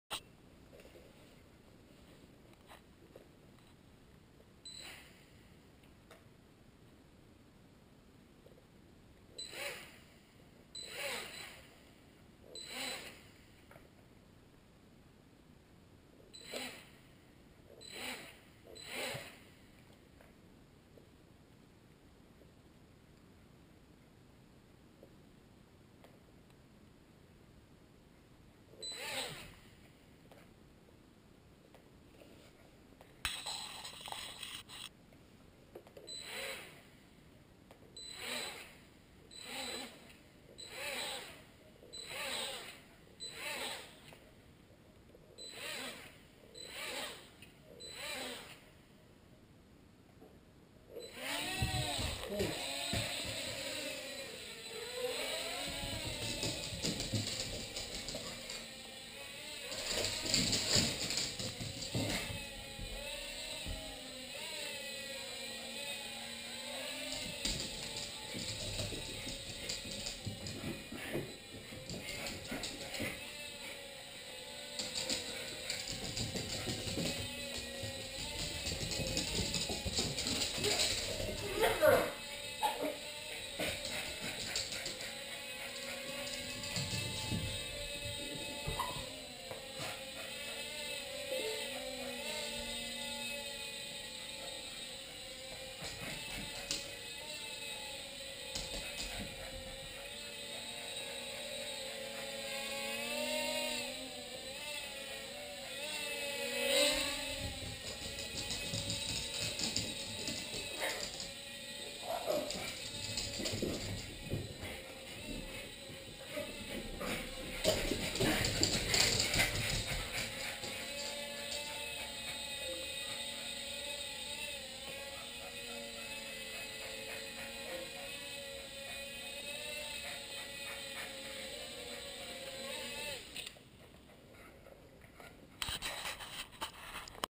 Boston Terrier Playing with a Quadcopter
Kori's found a new favorite way to get exercise inside the house!

Dog toy - Blade Nano QX Quadcopter